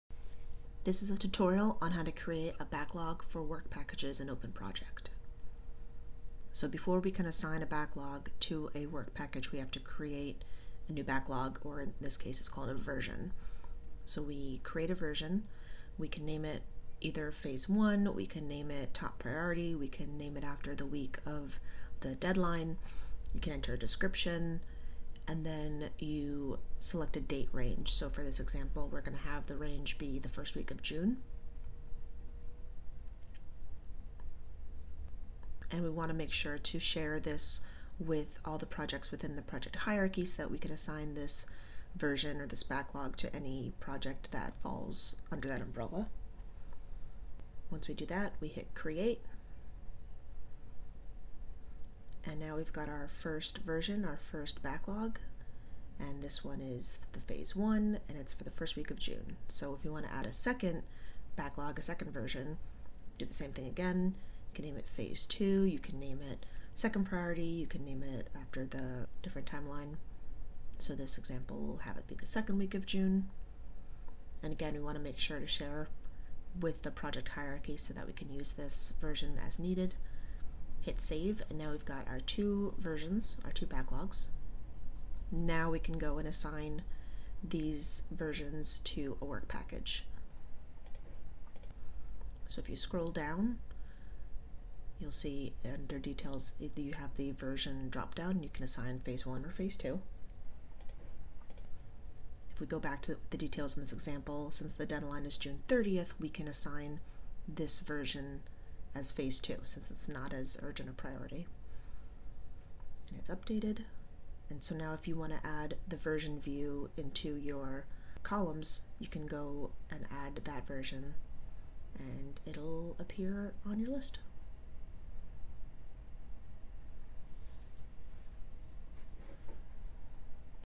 How to Create a Backlog in OpenProject
This is a mini-tutorial for how to create a Backlog in OpenProject